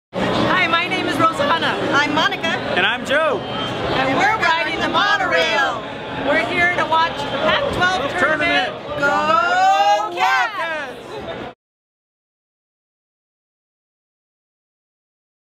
Las Vegas Travel Guide for Spring - Summer 2014 | Las Vegas Monorail Review
The Las Vegas Monorail is the best transportation to get you to all the top entertainment activities of the 2014 Spring - Summer season.  It is one of the most efficient and cheapest way to get around Las Vegas from Hotel/Casino to Hotel/Casino. In just 15 minutes or less, the Las Vegas Monorail connects you to top attractions, casinos, restaurants, shows, shops and resorts on the Las Vegas Strip. No waiting in lines, piling on buses or sitting in traffic making it the best way to get around the Las Vegas Strip.

The Las Vegas Monorail is a seven-stop, elevated train system that travels along a 3.9-mile route (6.4 kilometers) and connects major hotels and attractions along the world famous Las Vegas Strip. It enables passengers to travel the length of the resort corridor in 15 minutes or less in a safe, clean, comfortable and climate-controlled environment. With trains arriving every few minutes, passengers are quickly connected to world class resorts, hotels and casinos, shops, restaurants, entertainment venues and the Las Vegas Convention Center.

It also features the first fully-automated control network system for driverless operation and incorporates fail-safe protection throughout the software and equipment, making it one of the most technologically advanced urban transportation systems in the world.